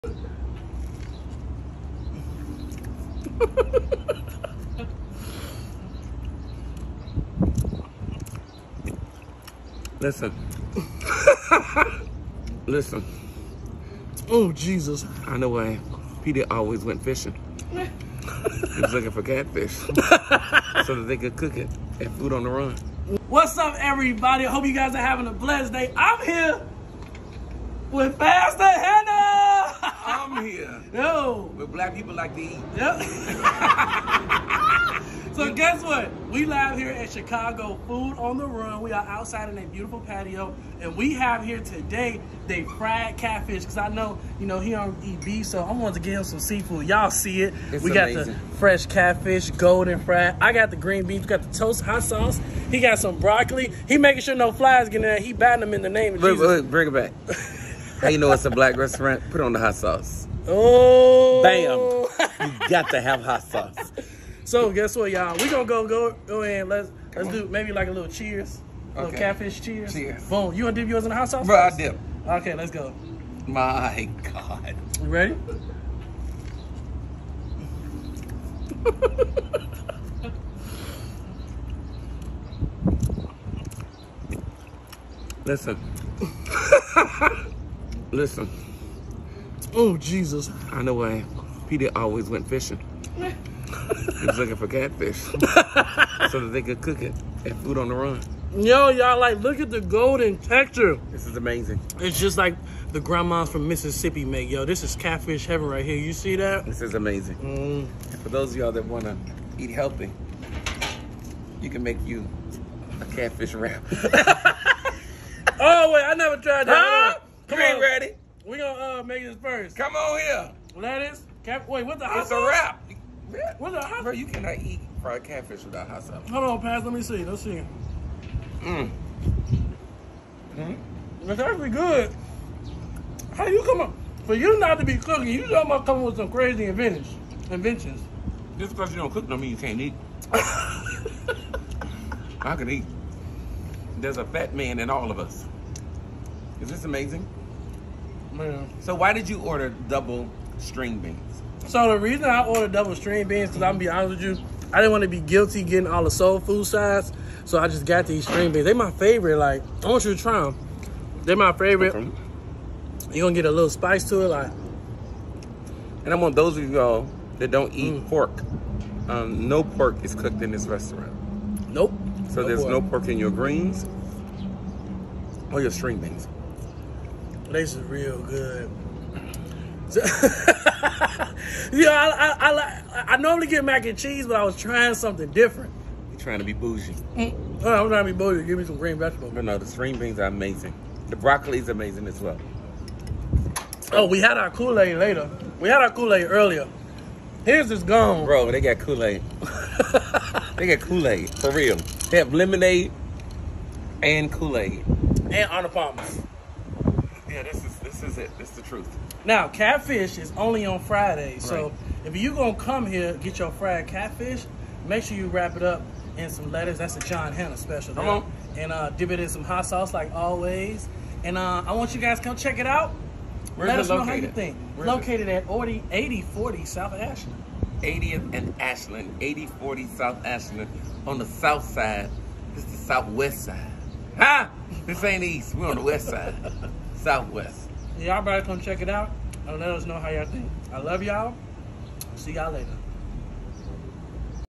Pastor John Hannah Tries Chicago Food On The Run Crispy Fried Catfish Mukbang Interview
Today Black People Eats had to stop by Chicago Food On The Run in Chicago, IL and we brought Pastor John Hannah with us to try their Pepper Steak Dinner! This dinner comes with tender juicy pepper steak, rice, bell peppers, green beans and candied yams.

Located at 8040 S Ashland, Chicago, IL 60620

Mention/Use blackpeopleeats when taking pictures/videos

We enjoyed the food, environment, and experience of this amazing restaurant that you must experience! I hope you all check out this restaurant the next time you're in Chicago!

✔️Connecting people to black-owned food & beverage companies in your city‼️